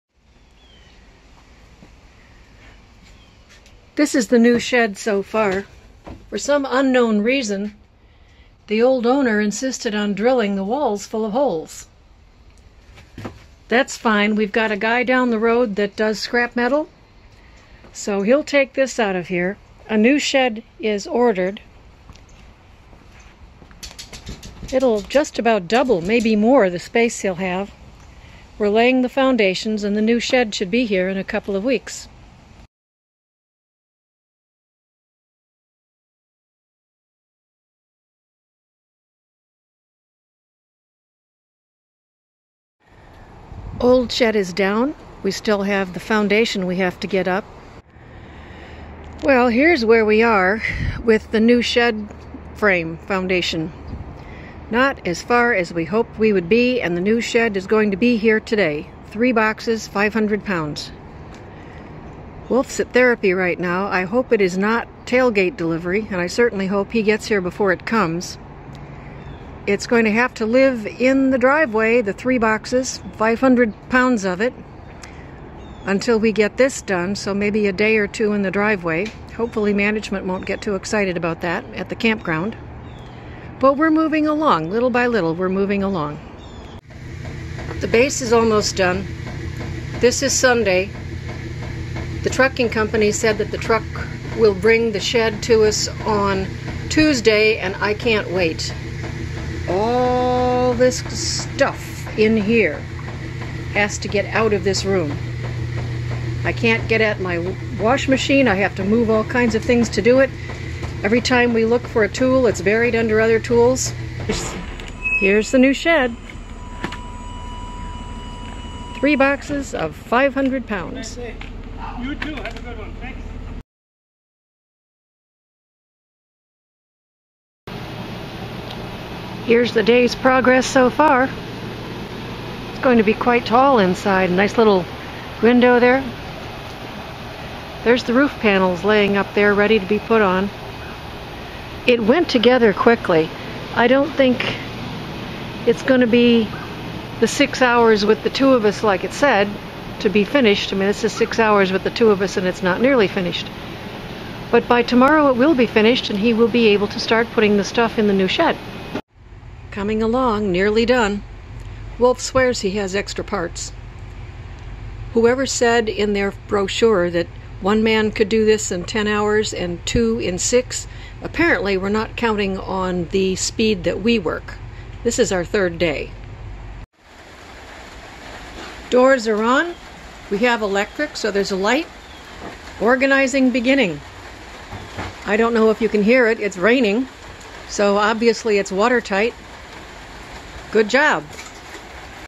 Shedding the Old Shed
The big thing about the casa which bothered Wolf was the old shed. It was way too small and was riddled with holes. So, out with the old shed and in with the new shed. It is a Keter, very easy to assemble, with the walls tongue and groove, and the roof parts having well fitted slots.